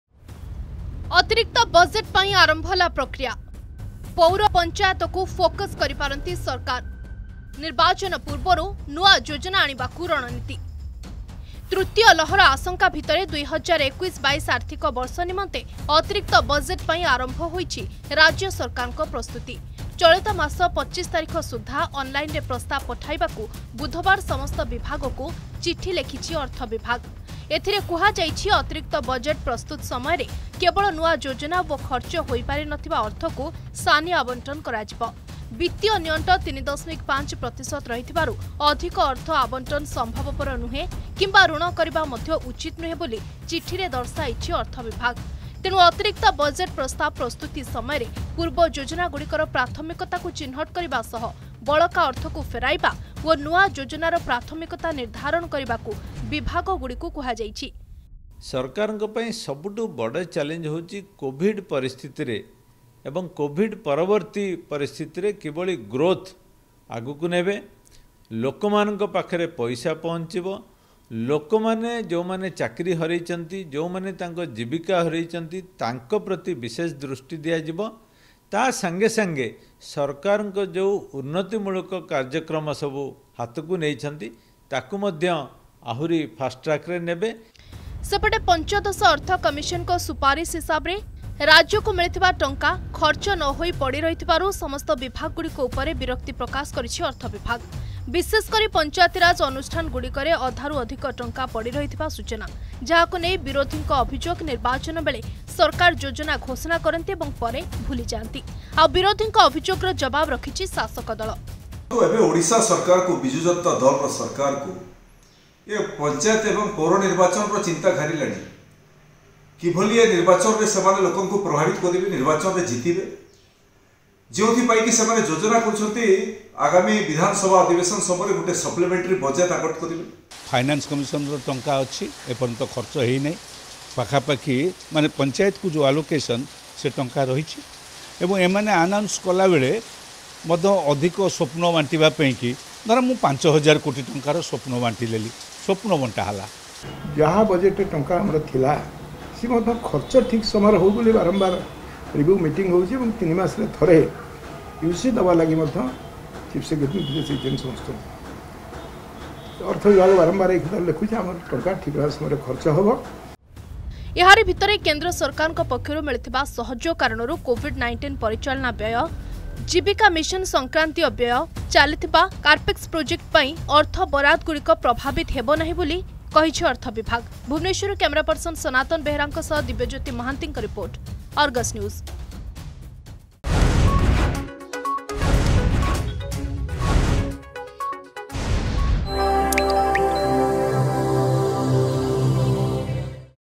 ଅତିରିକ୍ତ ବଜେଟ ପାଇଁ ଆରମ୍ଭ ହେଲା ପକ୍ରିୟା
ତୃତୀୟ ଲହର ଆଶଙ୍କା ମଧ୍ୟରେ ୨୦୨୧-୨୨ ଆର୍ଥିକ ବର୍ଷ ନିମନ୍ତେ ଅତିରିକ୍ତ ବଜେଟ୍ ପାଇଁ ଆରମ୍ଭ ହୋଇଛି ରାଜ୍ୟ ସରକାରଙ୍କ ପ୍ରସ୍ତୁତି । ଚଳିତମାସ ୨୫ତାରିଖ ସୁଦ୍ଧା ଅନଲାଇନରେ ପ୍ରସ୍ତାବ ପଠାଇବାକୁ ବୁଧବାର ସମସ୍ତ ବିଭାଗକୁ ଚିଠି ଲେଖିଛି ଅର୍ଥ ବିଭାଗ ।